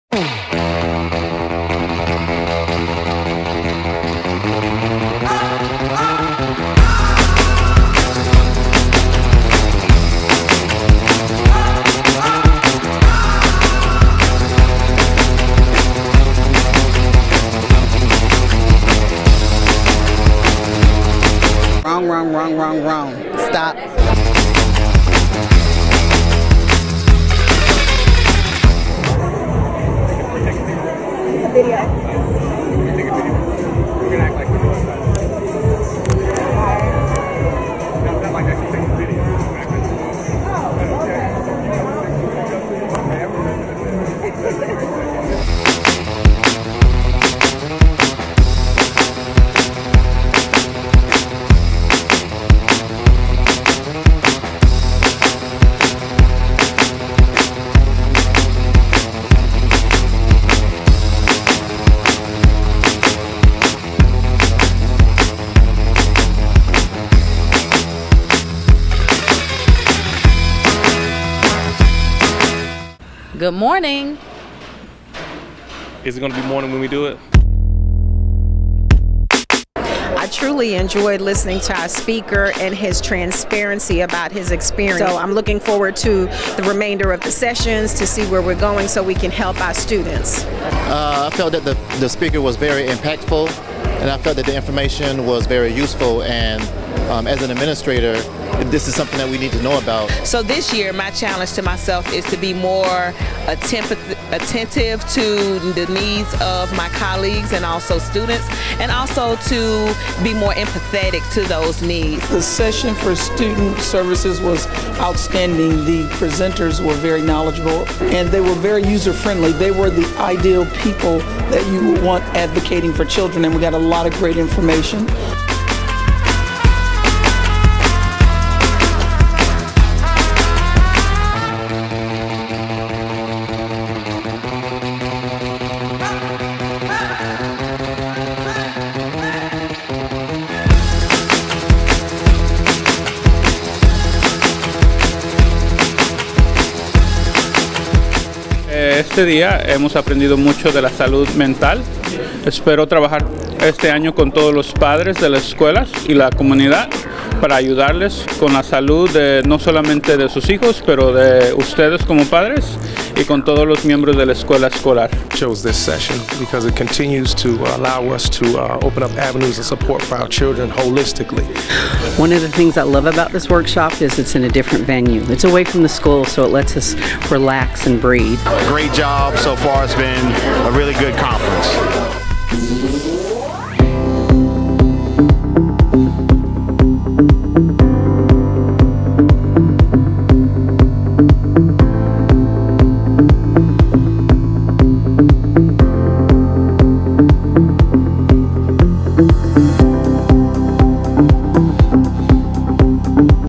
Summer Leadership Highlights 2019
Principals are getting ready for another great school year as they attend Summer Leadership 2019!